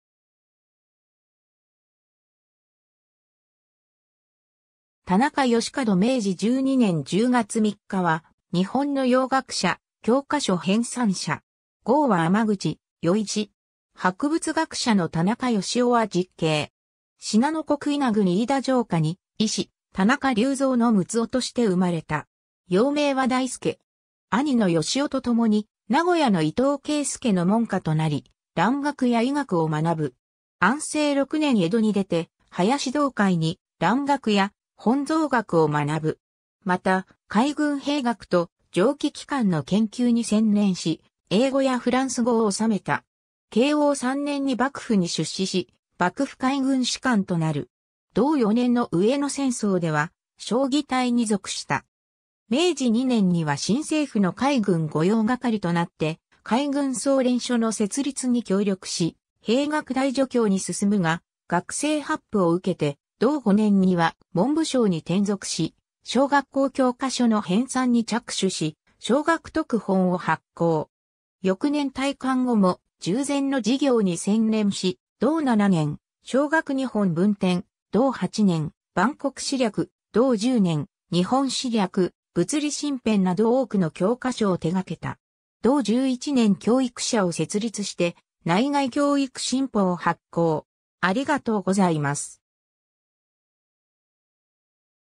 田中義廉
田中 義廉（たなか よしかど、天保12年2月11日（1841年4月2日） - 明治12年（1879年）10月3日）は日本の洋学者、教科書編纂者。
号は天口酔史。
博物学者の田中芳男は実兄。
 信濃国伊那郡飯田城下（現在の長野県飯田市）に医師・田中隆三（如水）の六男として生まれた。
幼名は大輔。
兄の芳男とともに名古屋の伊藤圭介の門下となり、蘭学や医学を学ぶ。
安政6年（1859年）江戸に出て、林洞海に蘭学や本草学を学ぶ。
また海軍兵学と蒸気機関の研究に専念し、英語やフランス語を修めた。
慶応3年（1867年）に幕府に出仕し、幕府海軍士官となる。
同4年（1868年）の上野戦争では彰義隊に属した。
明治2年（1869年）には新政府の海軍御用掛となって海軍操練所の設立に協力し、兵学大助教に進むが、学制発布を受けて、同5年（1872年）には文部省に転属し、小学校教科書の編纂に着手し、「小学読本」を発行。
翌年（1873年）退官後も、従前の事業に専念し、同7年（1874年）「小学日本文典」、同8年（1875年）「万国史略」、同10年（1877年）「日本史略」「物理新編」など多くの教科書を手掛けた。
同11年（1878年）教育社を設立して「内外教育新報」を発行。